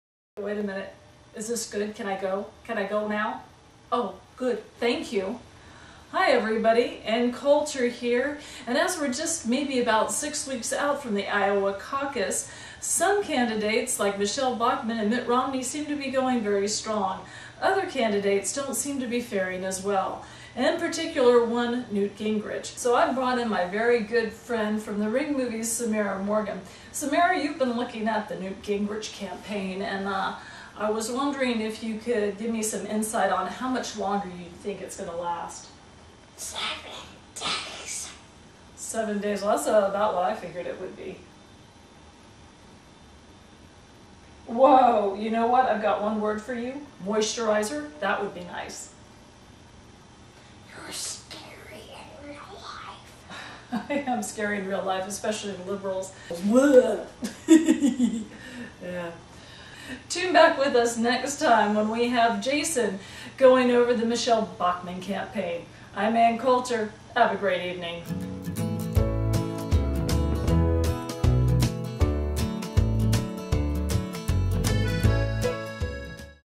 Ann Coulter meets Samera Morgan
Conservative cougar Anne Coulter gives her insight on the Repubilican primaries with a special guest commentator.